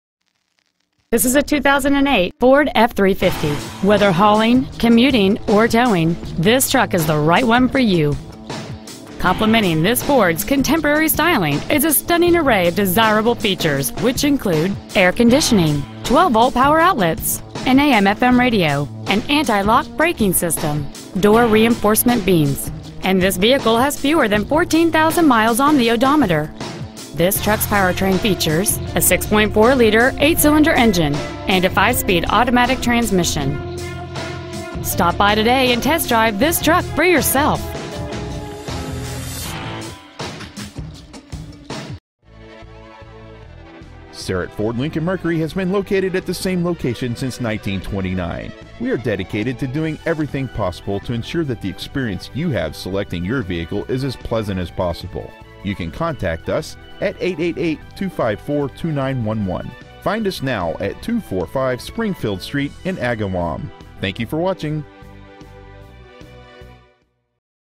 Pre-Owned 2008 FORD F-350 Enfield CT
We've been honored to serve the Agawam  MA area, we promise that your experience at our dealership will exceed your expectations!

 Year: 2008
 Make: FORD
 Model: F-350
 Engine: 6.4L V8
 Trans.: AUTO 5SPD
 Exterior: BLACK
 Miles: 13,070
 Interior: CAMEL

 Pre-Owned 2008 FORD F-350 Agawam  MA